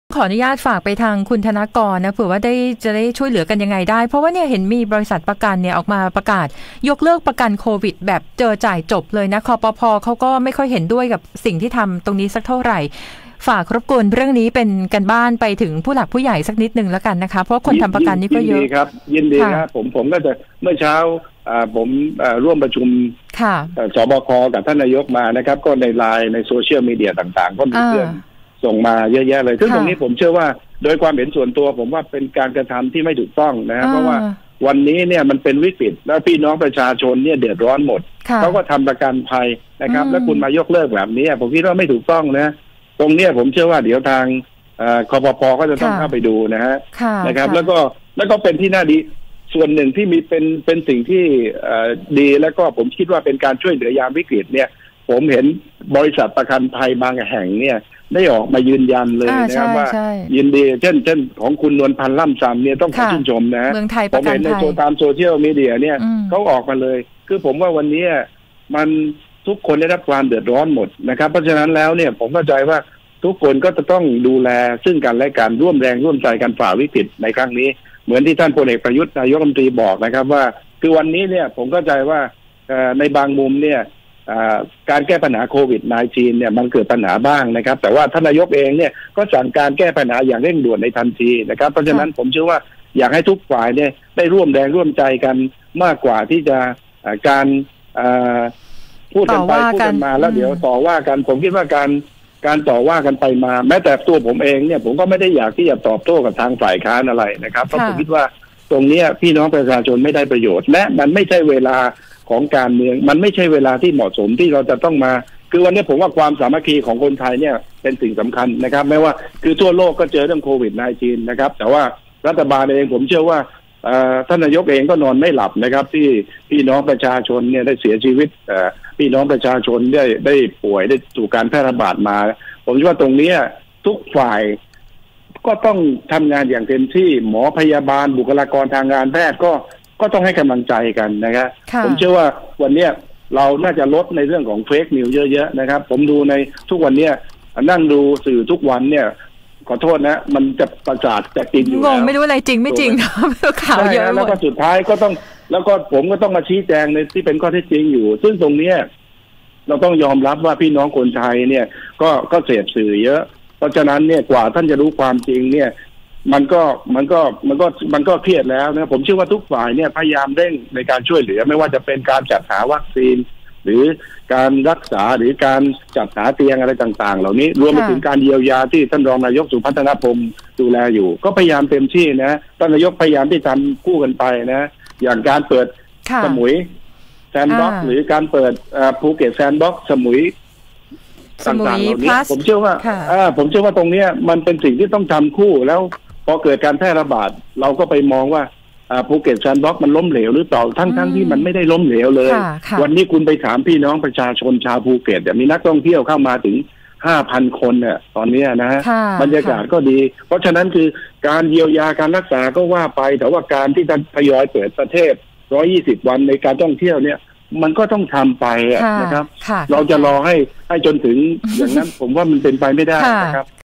การแก้ปัญหาบริษัทประกันภัย ยกเลิกสัญญาประกันโควิด-19
สัมภาษณ์ นายธนกร วังบุญคงชนะ เลขานุการรัฐมนตรีประจำสำนักนายกรัฐมนตรี ในฐานะโฆษกฯ ศบศ. การแก้ปัญหาบริษัทประกันภัย ยกเลิกสัญญาประกันโควิด
-19